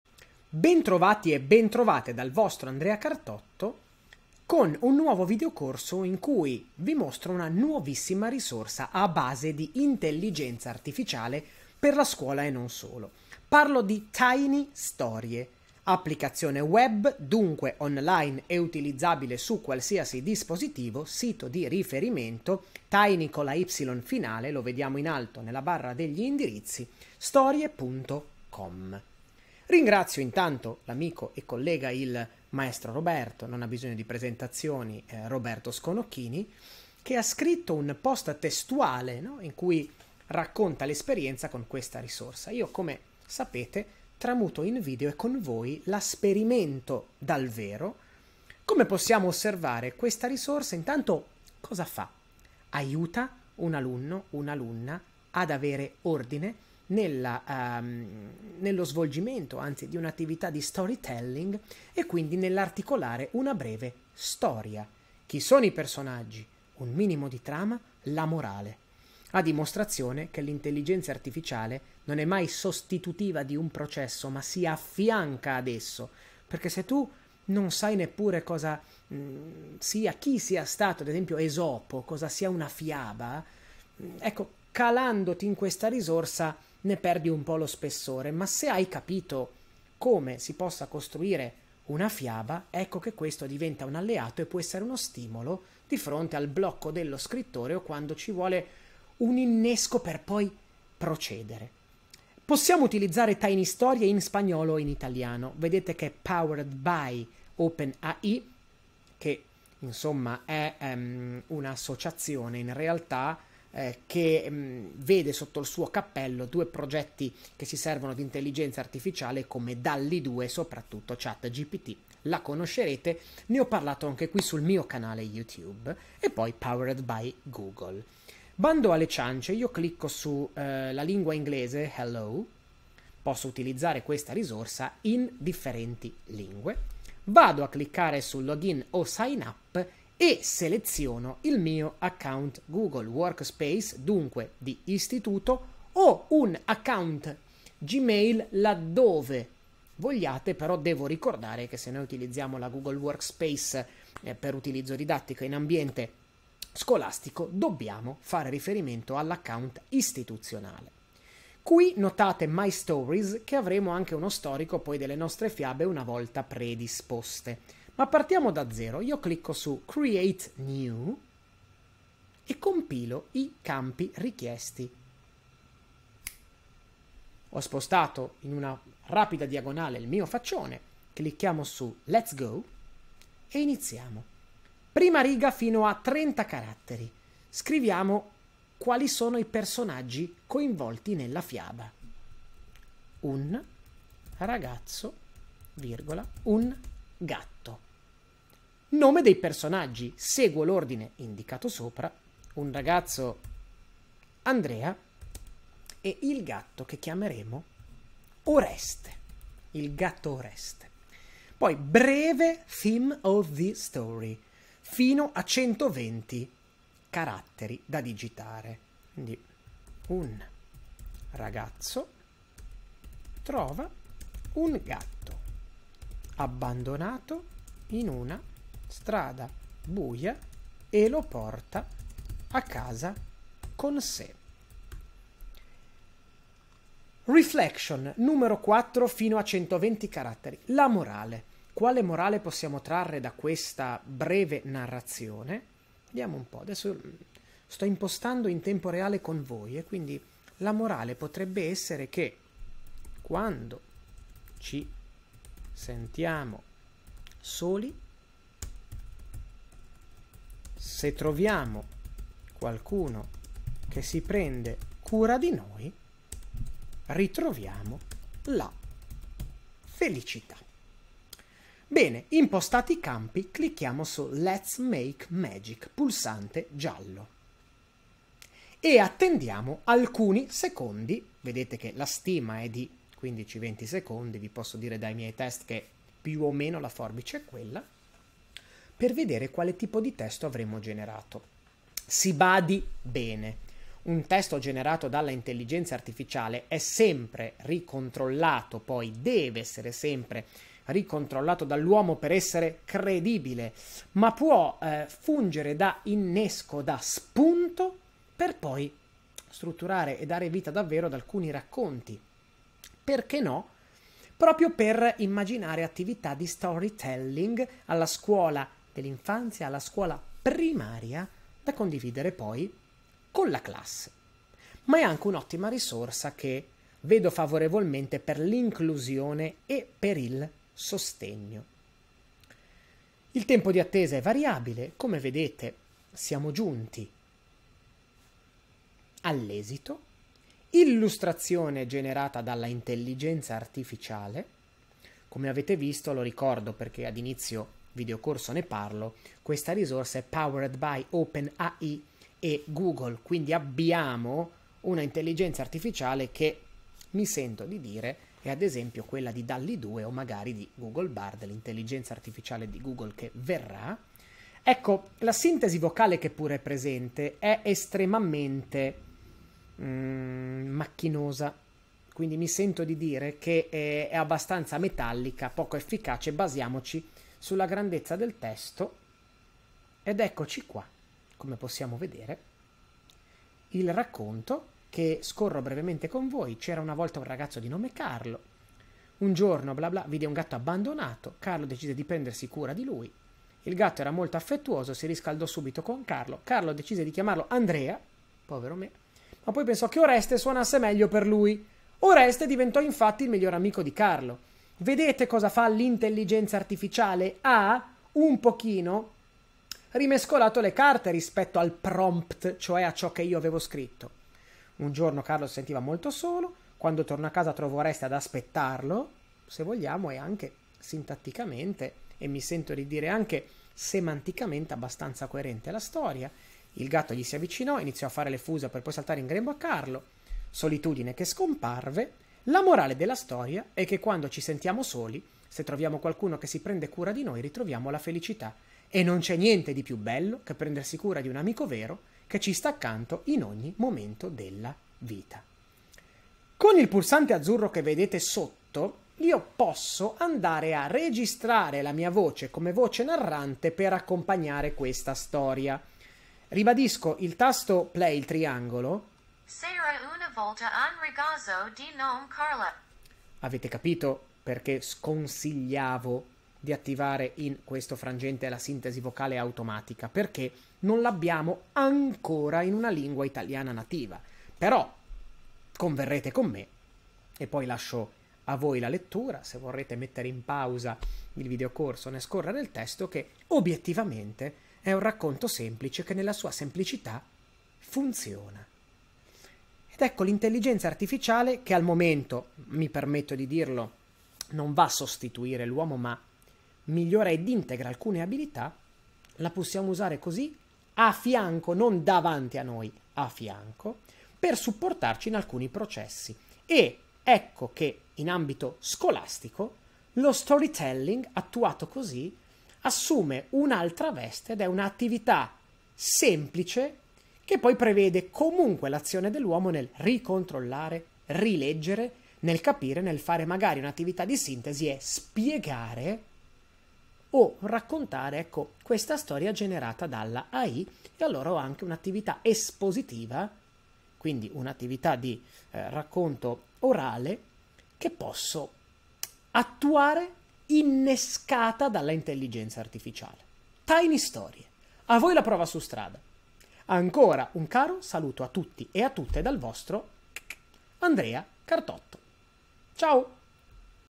Andrea Cartotto - Lo storytelling per i più piccoli a base di AI: scopriamo TinyStorie!
Torno ad occuparmi di Intelligenza Artificiale nel suo utilizzo didattico, a Scuola, strizzando l'occhio ai più piccoli: grazie, infatti, alla nuovissima e gratuita applicazione web Tiny Storie è possibile "raccontare una storia" (attività di storytelling) in maniera diversa e sfruttando una tecnologia che si basa sull'intelligenza artificiale. Docenti ed alunni potranno pensare ad un racconto...e vederlo quasi per magia illustrato e trascritto! Fantasia e creatività non potranno mancare per utilizzare questa app, così come sarà sempre indispensabile una adeguata rilettura ma...rimarrete, comunque, a bocca aperta! ;-) Come sempre, graditi/e feedbacks e condivisioni!